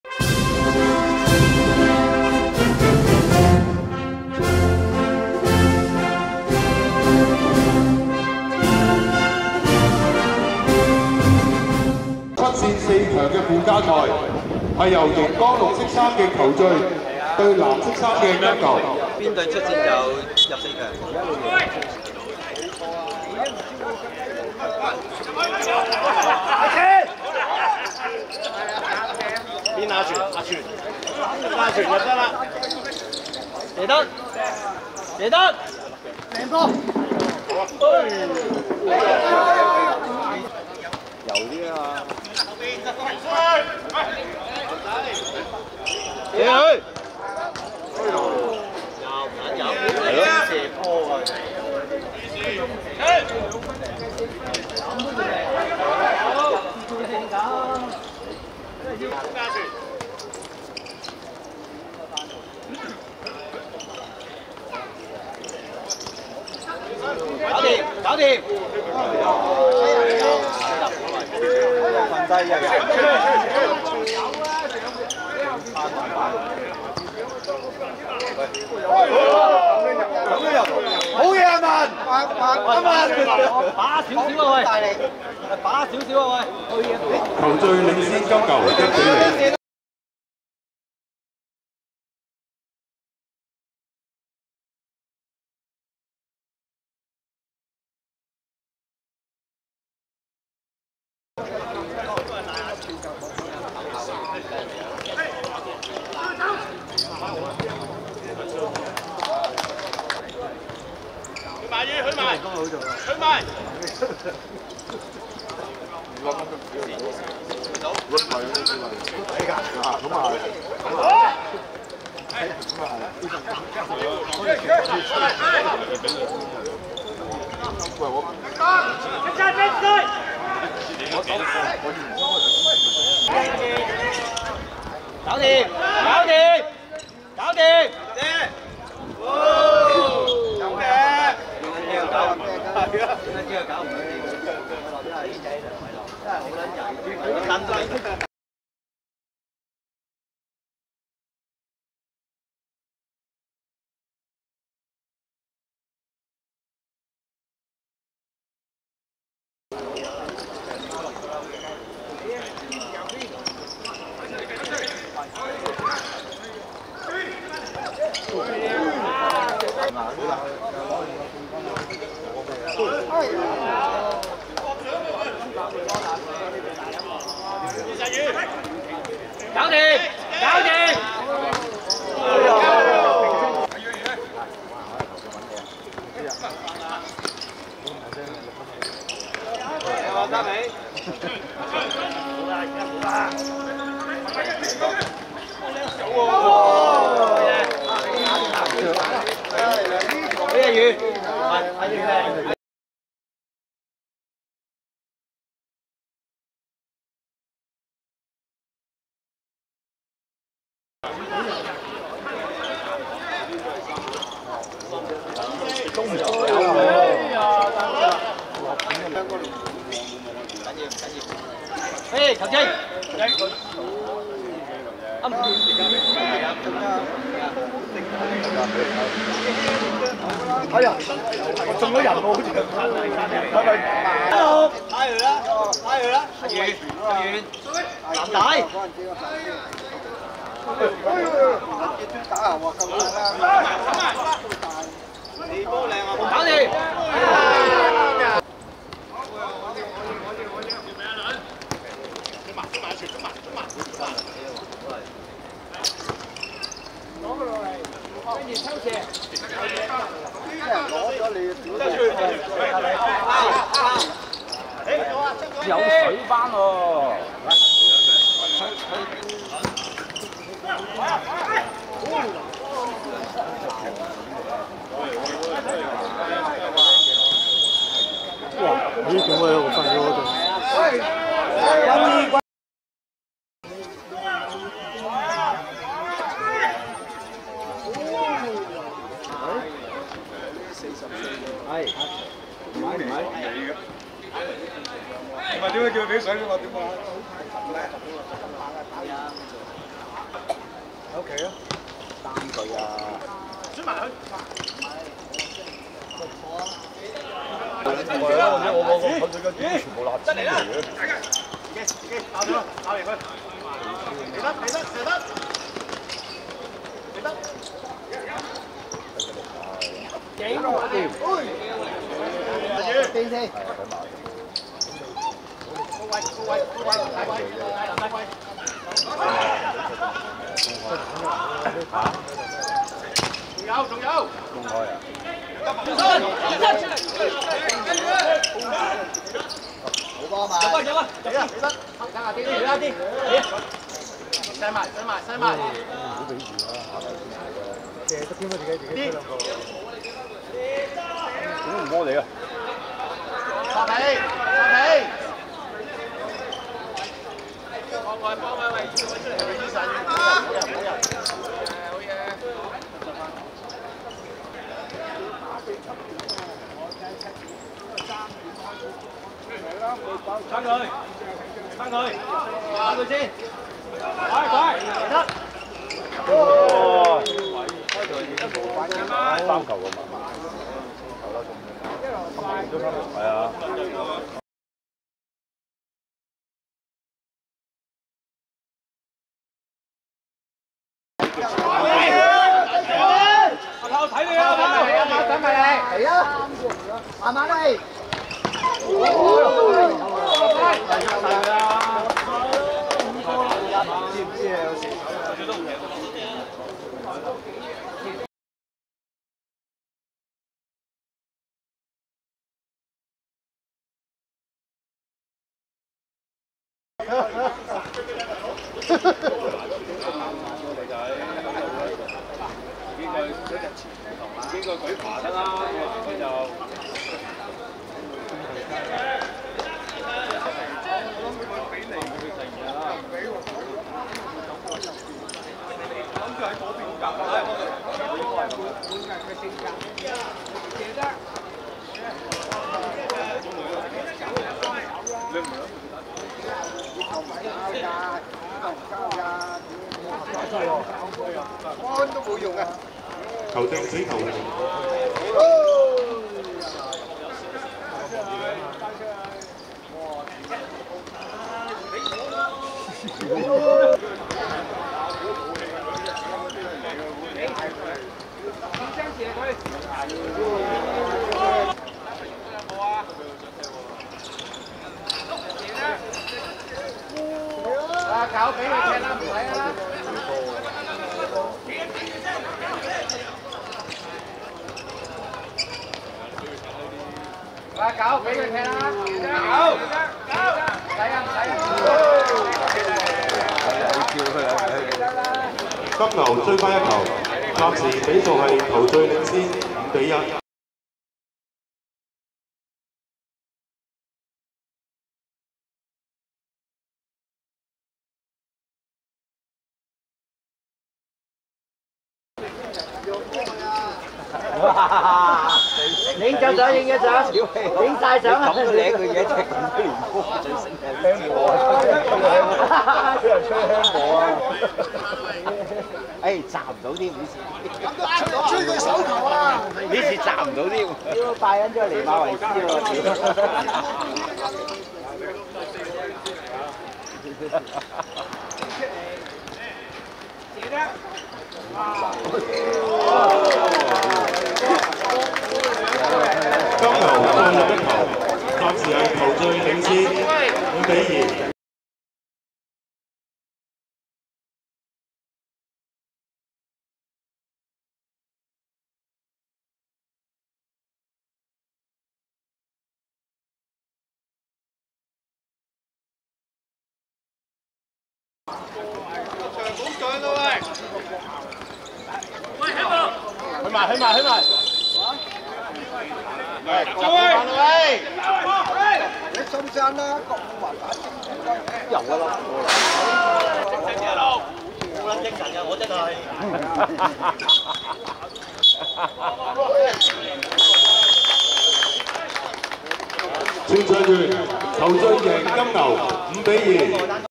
球醉vs金牛(2023.10.16.元朗屯門七鄉國慶盃七人足球邀請賽~40歳組附加賽)精華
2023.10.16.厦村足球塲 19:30
球醉(青) 5 : 2 金牛(藍)